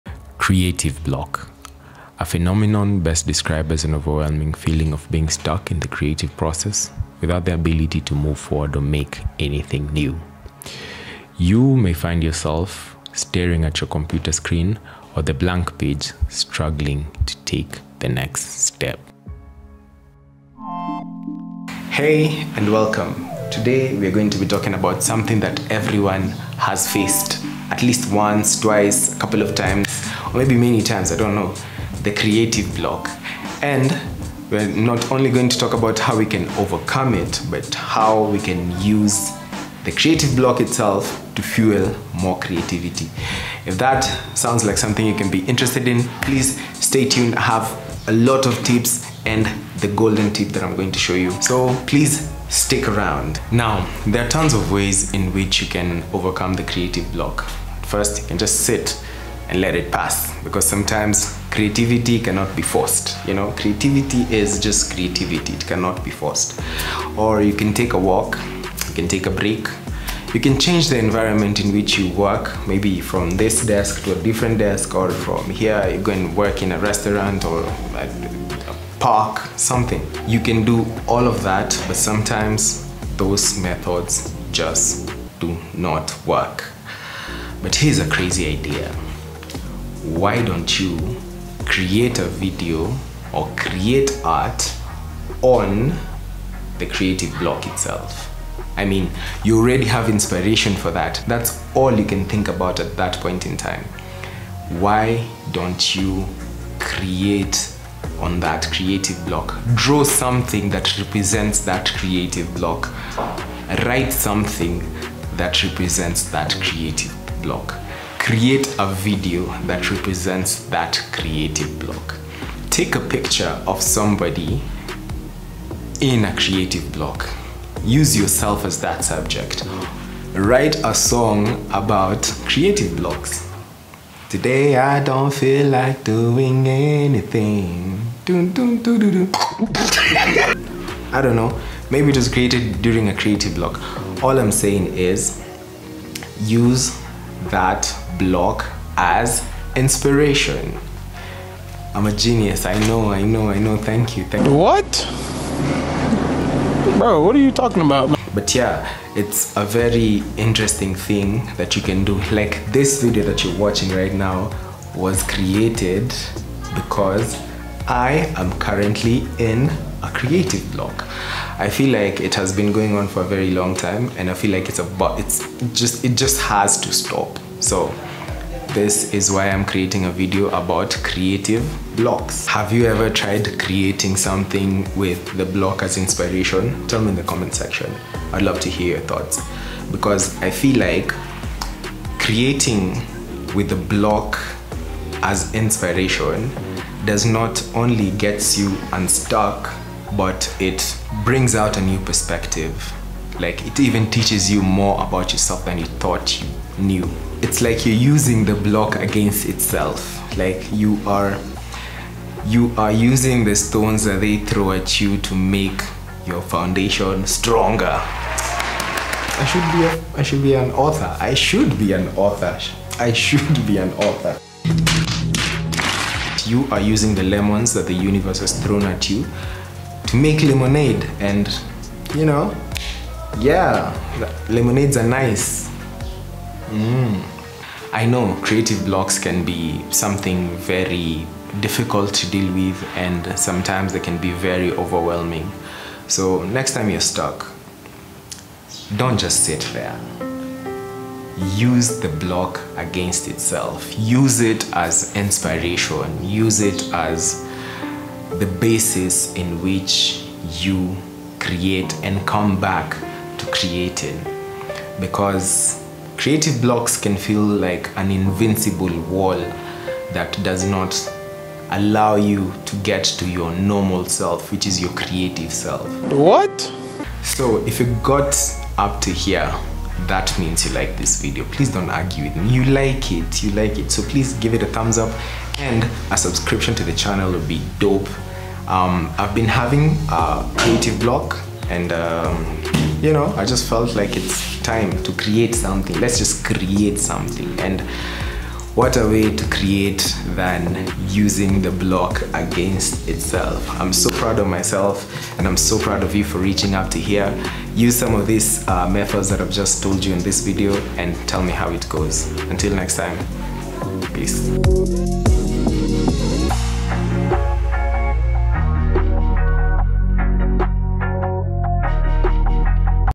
How to overcome Creative Block
Feeling stuck? In this video, I’ll show you how to overcome creative block and how your block can actually be the key to unlocking your best ideas! Discover unique techniques to transform your block into a source of inspiration. I’ll share practical tips and personal experiences that reveal how embracing your struggles can lead to creative breakthroughs. Don’t let your blocks hold you back—learn how to turn them into your greatest asset!

Join me as we explore this powerful approach to creativity and find out how to use your blocks as a springboard for new ideas. If you’ve ever felt stuck, this video is for you!